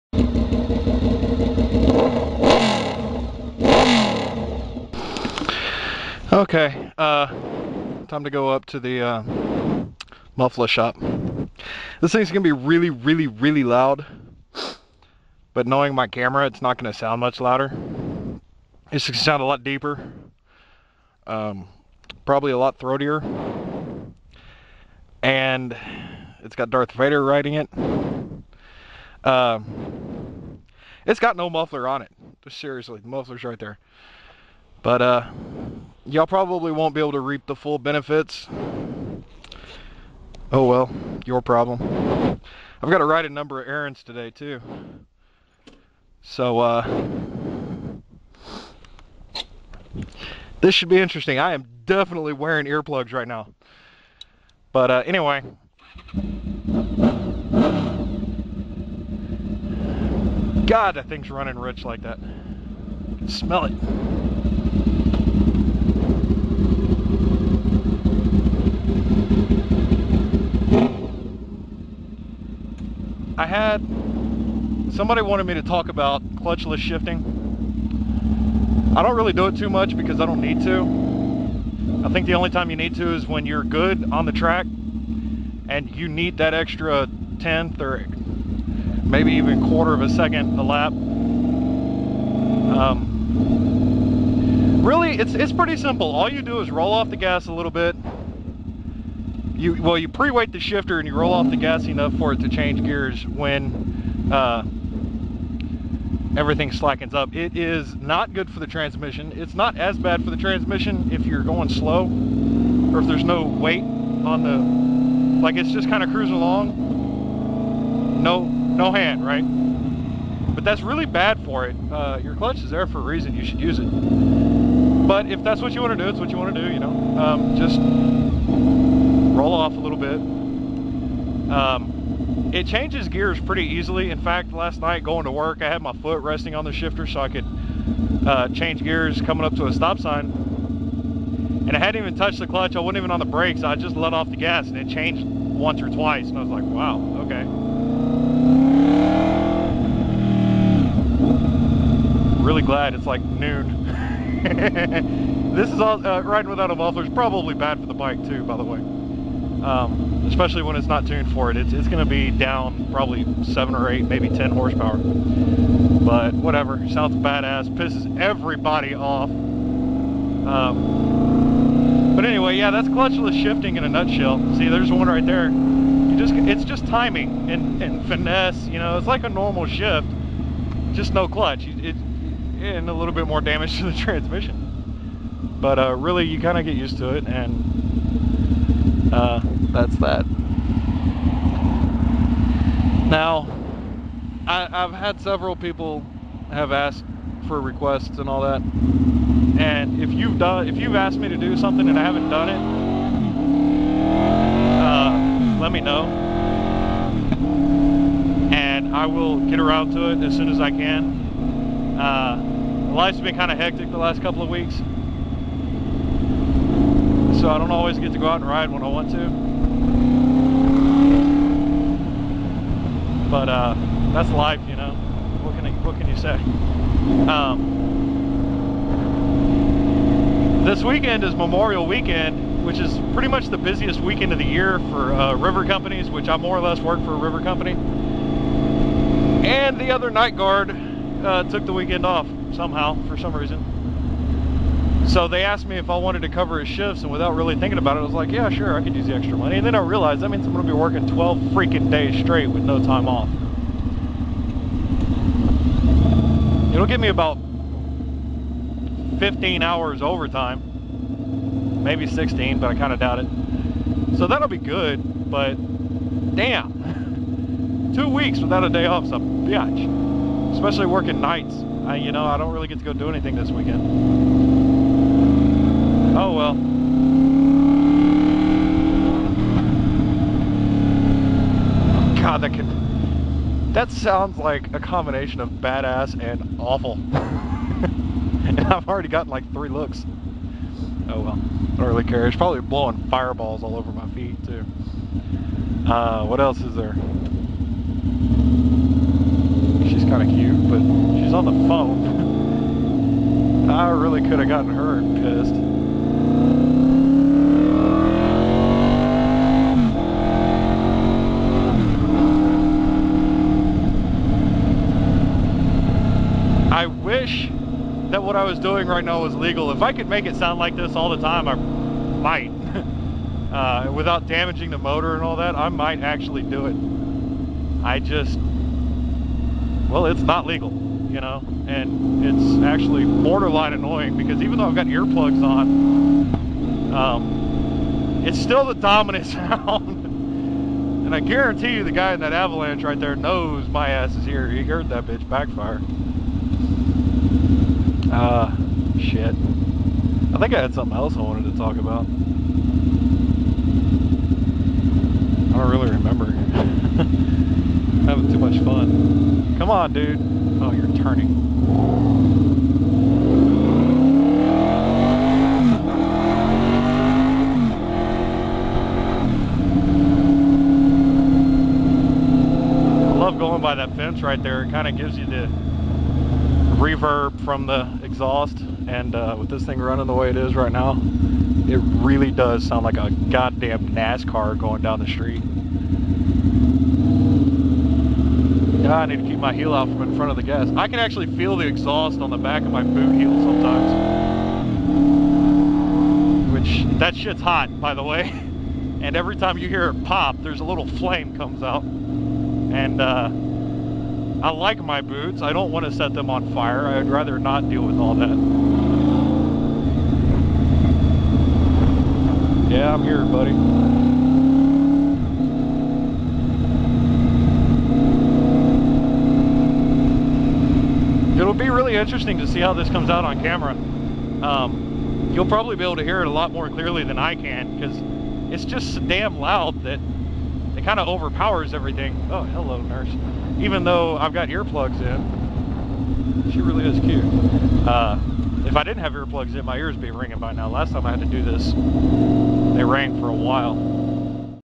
Heading to the muffler shop
Had to go get a little tab welded on for the center stand. Someone wanted me to talk about shifting without the clutch, so I do, but it's pretty brief as it's hard to actually explain, much easier to deal with in person. There's a few random other topics, but it's mostly me being overcome with noise by riding with open headers. :)